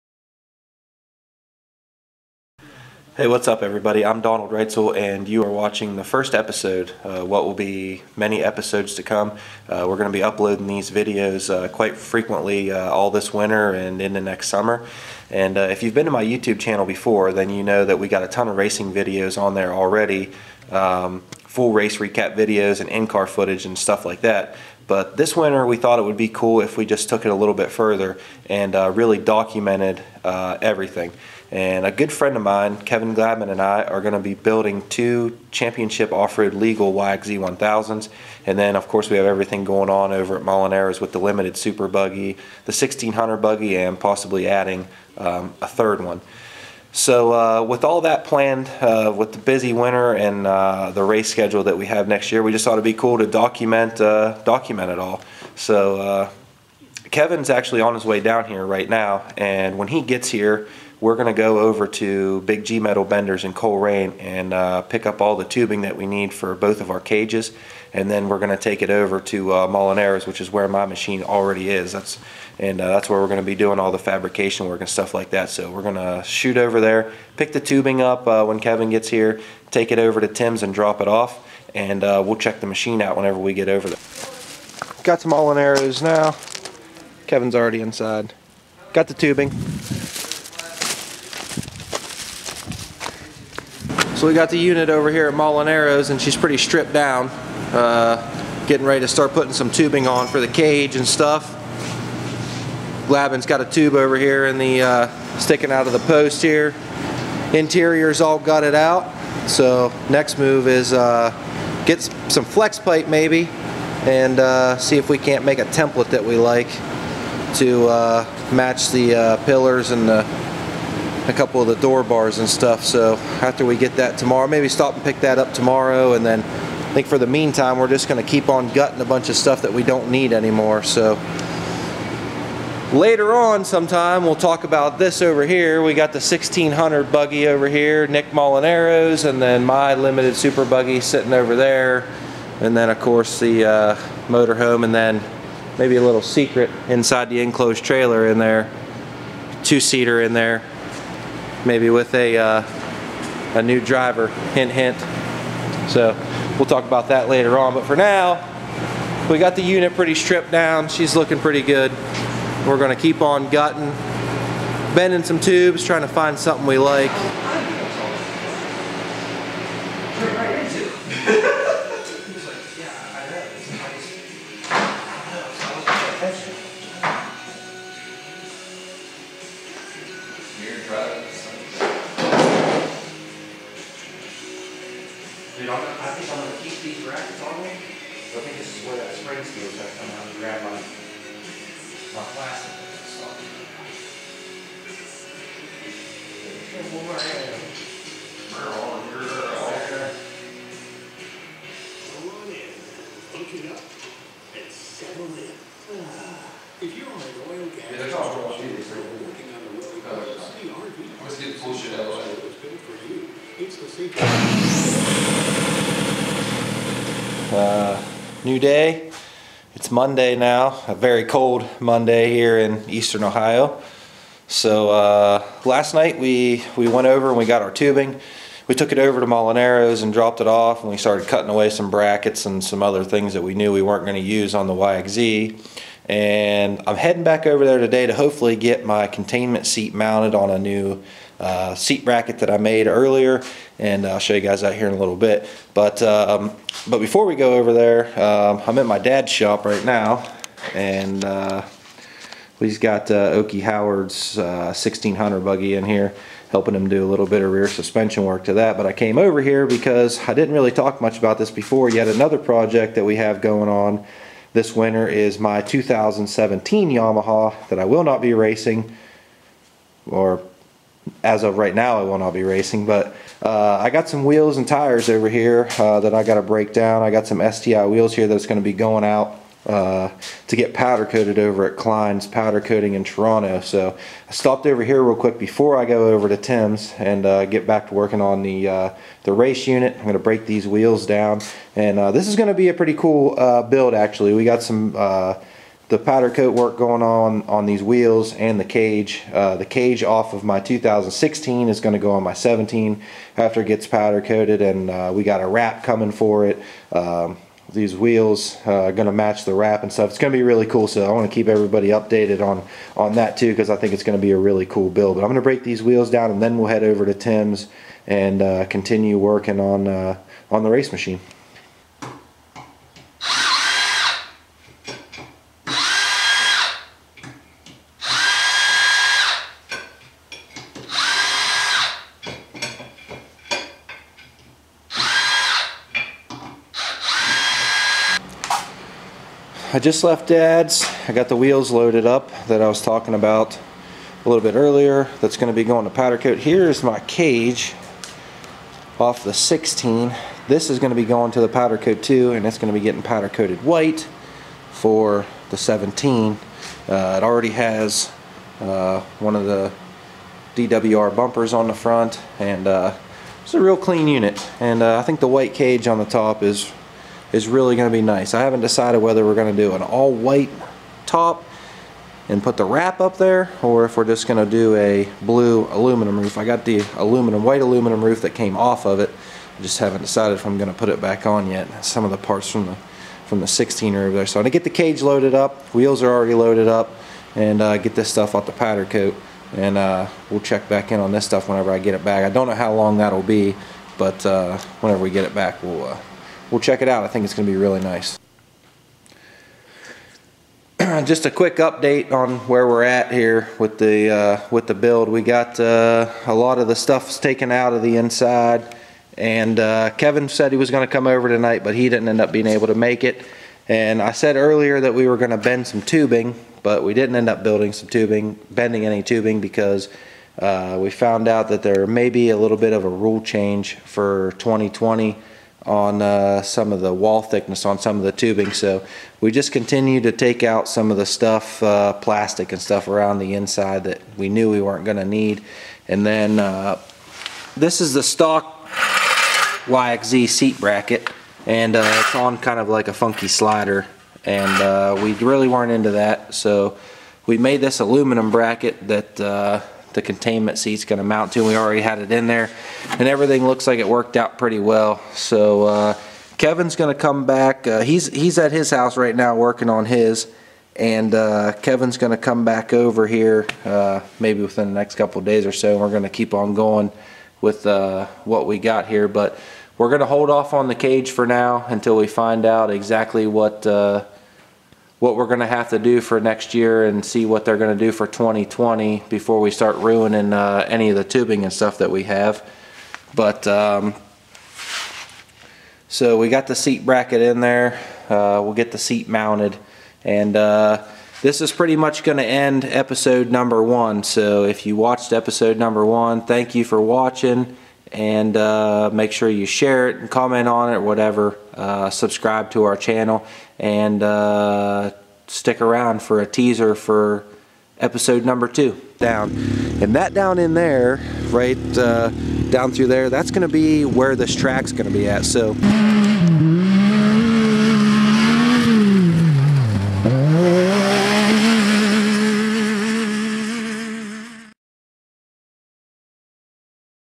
Episode 1. We start building 1 of 2 COR legal YXZs
Episode 1 The intro to our new series. We get started tearing down the first of 3 YXZs that we will be building and we talk a little bit about what this series of videos will be all about.